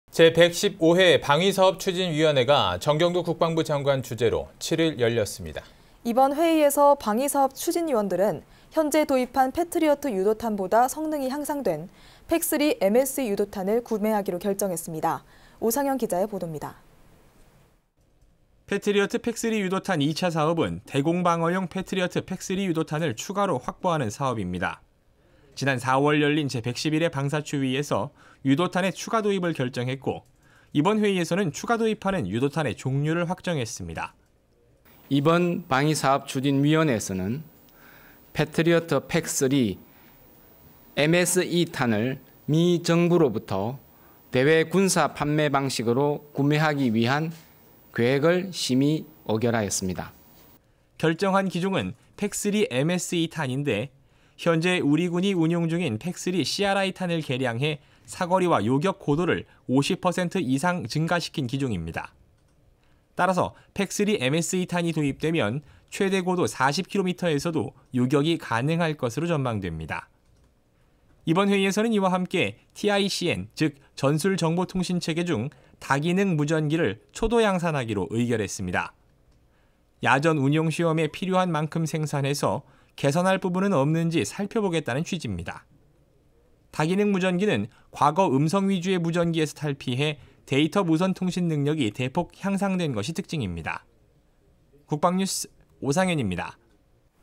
[국방뉴스]18.11.08 제115회 방위사업추진위원회, 패트리어트 PAC-3 MSE 유도탄 구매 결정
강경일)
제115회 방위사업추진위원회가 정경두 국방부장관 주재로 7일 열렸습니다.
심예슬)
이번 회의에서 방위사업추진위원들은 현재 도입한 패트리어트 유도탄보다 성능이 향상된 PAC-3 MSE 유도탄을 구매하기로 결정했습니다. 오상현 기자의 보돕니다.

패트리어트 PAC-3 유도탄 2차 사업은 대공 방어용 패트리어트 PAC-3 유도탄을 추가로 확보하는 사업입니다. 지난 4월 열린 제111회 방사추위에서 유도탄의 추가도입을 결정했고 이번 회의에서는 추가도입하는 유도탄의 종류를 확정했습니다.

SOV. 강환석 방위사업청 대변인
“이번 방위사업추진위원회에서는 패트리어트 PAC-3 MSE탄을 미 정부로부터 대외군사판매 방식으로 구매하기 위한 계획을 심의 의결했습니다. ”

결정한 기종은 PAC-3 MSE 탄인데 현재 우리 군이 운용중인 PAC-3 CRI탄을 개량해 사거리와 요격고도를 50%이상 증가시킨 기종입니다. 따라서 PAC-3 MSE탄이 도입되면 최대 고도 40km에서도 요격이 가능할 것으로 전망됩니다. 이번 회의에서는 이와함께 TICN 즉 전술정보통신체계 중 다기능 무전기를 초도양산하기로 의결했습니다. 야전운용시험에 필요한 만큼 생산해 개선할 부분은 없는 지 살펴보겠다는 취집니다. 다기능 무전기는 과거 음성 위주의 무전기에서 탈피해 데이터 무선통신능력이 대폭 향상된 것이 특징입니다.

국방뉴스 오상현입니다.